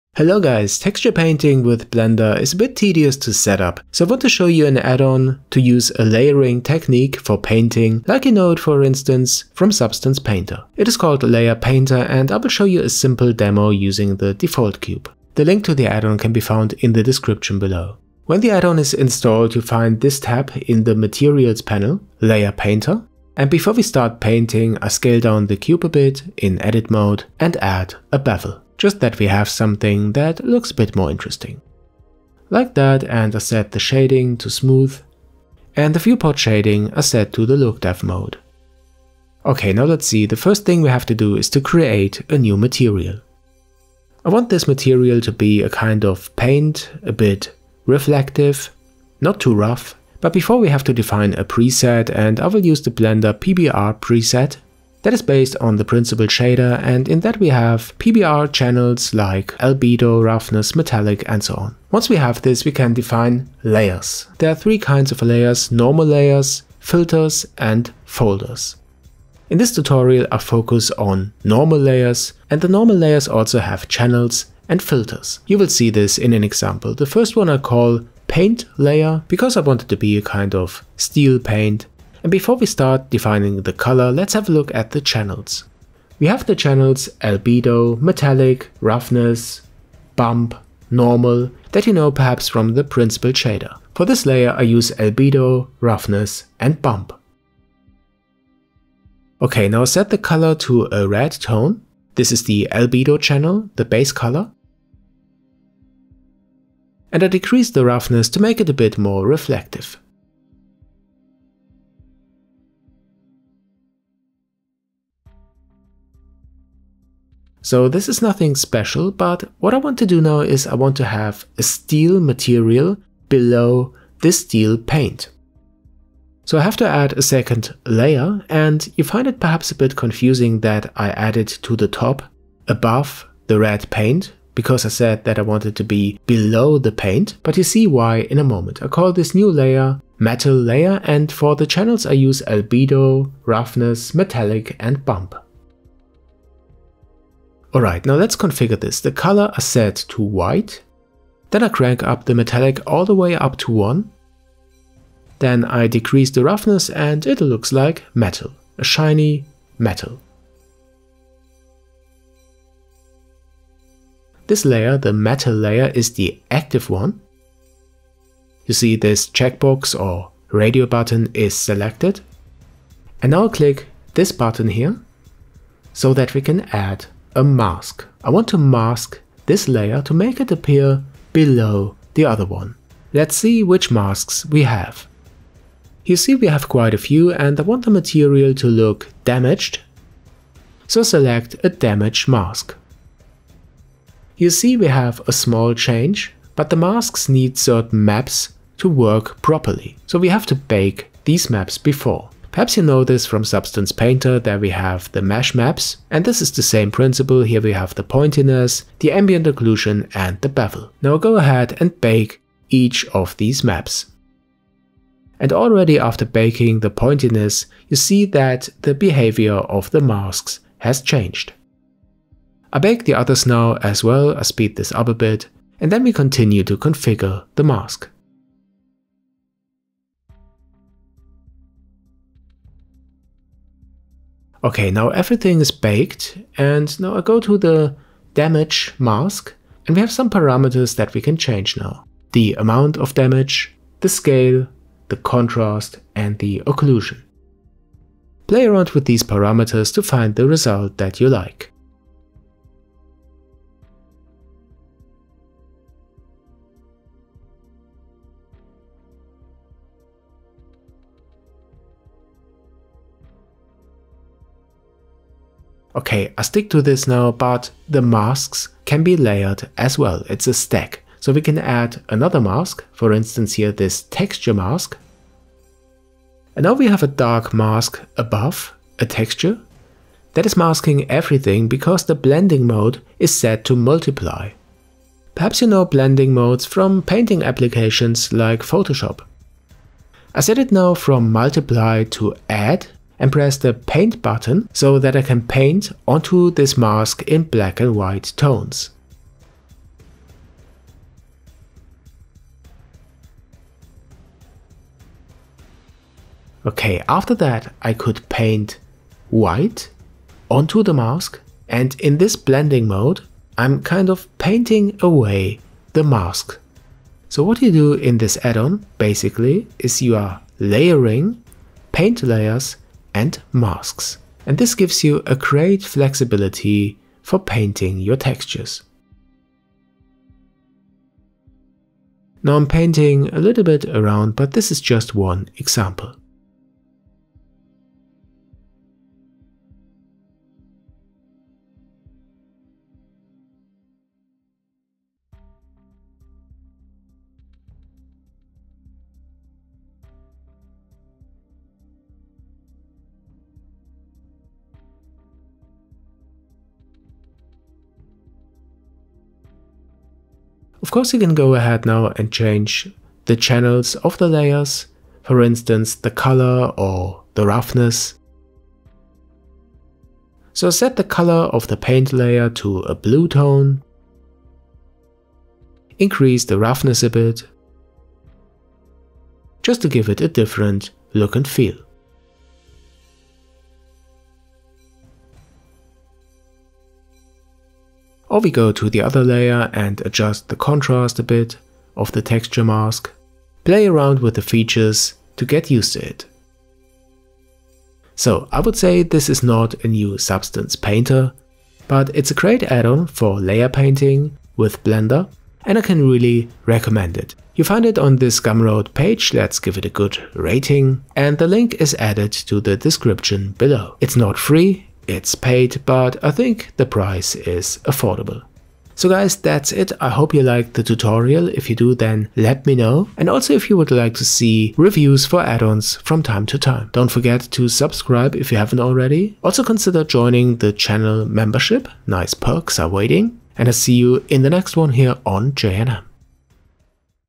Blender Texture Painting : Layer Painter Tutorial
Blender 2.8 and 2.9 offers great features for texture painting, but no layering approach like you know it from Substance Painter. In this video I show an addon called Layer painter that can be used to for texturing with PBR materials, layers and masks.